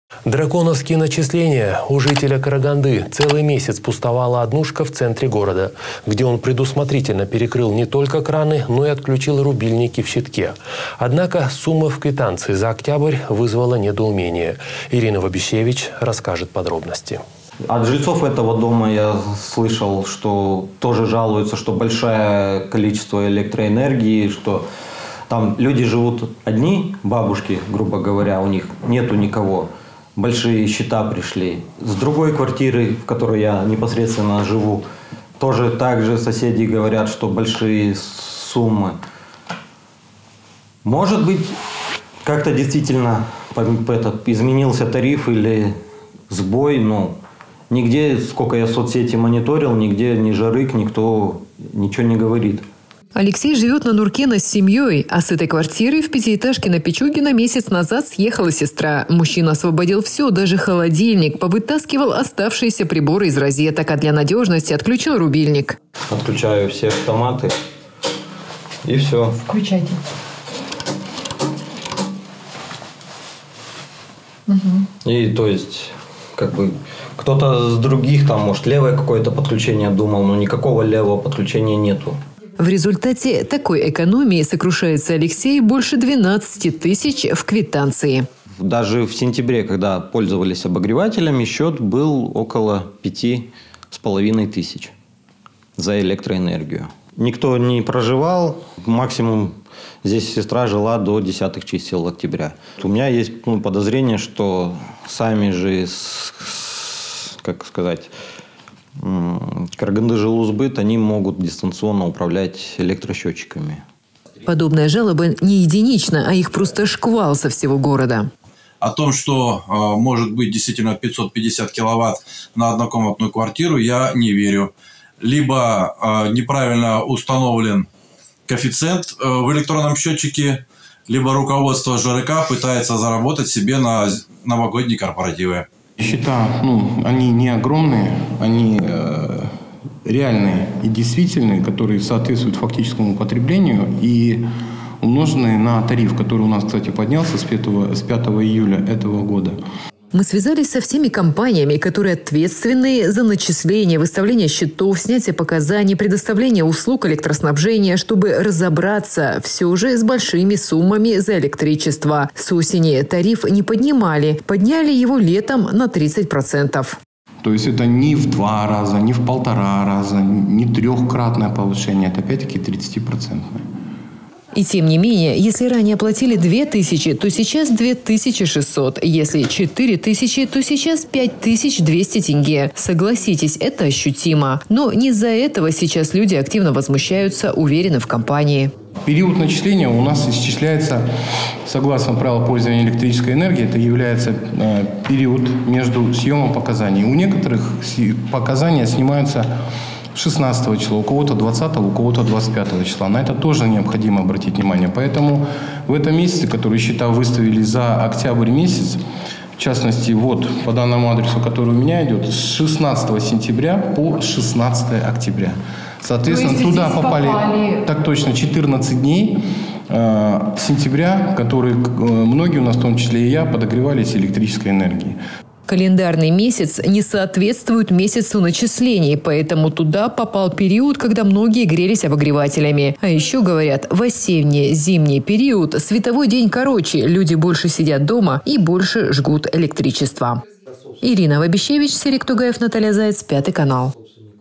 «Драконовские» начисления.
У жителя Караганды целый месяц пустовала «однушка» в центре города, где он предусмотрительно перекрыл не только краны, но и отключил рубильники в щитке. Однако сумма в квитанции за октябрь вызвала недоумение!